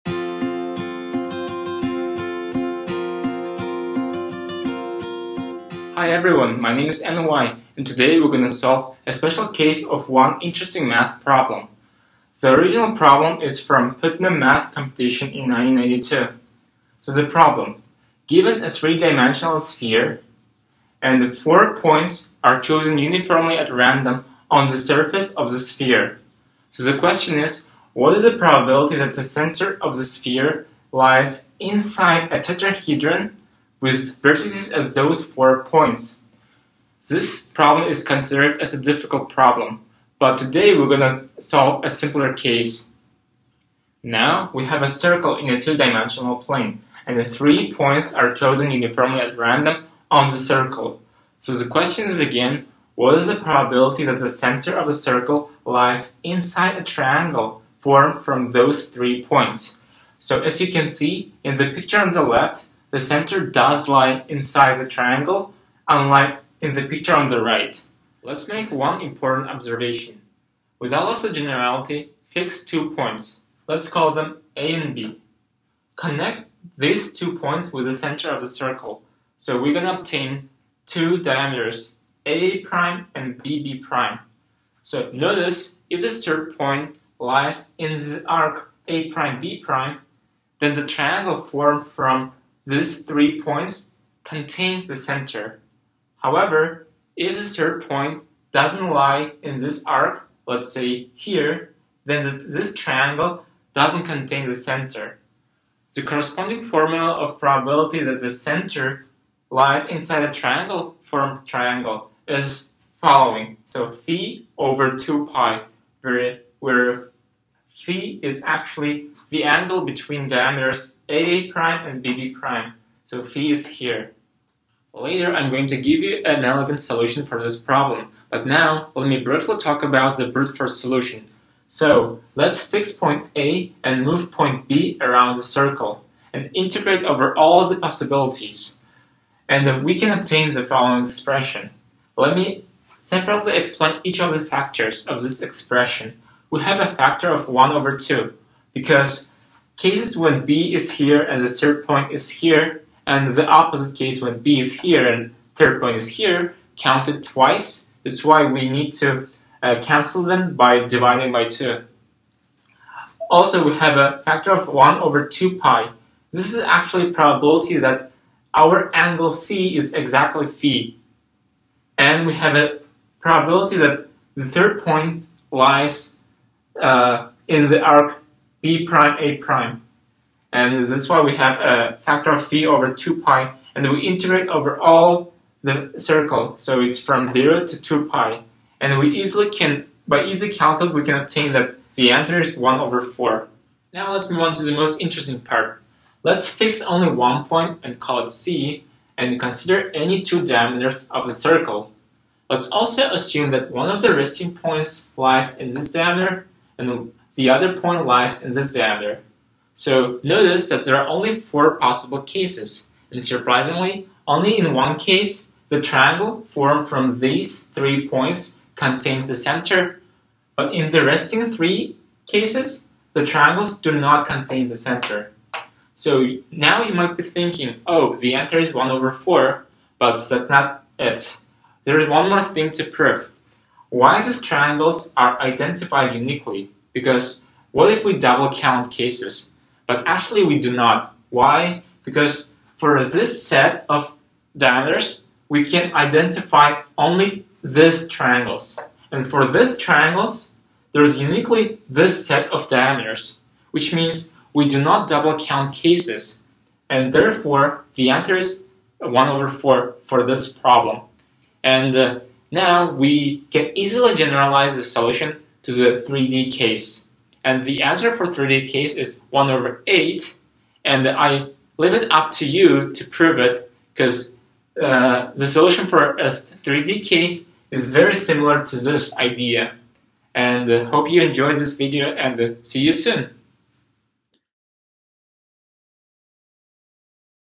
An interesting problem about probabilities in Geometry
This short five-minute video about  a technical concept made for an academic student position. Note that this video is not for commercial purposes.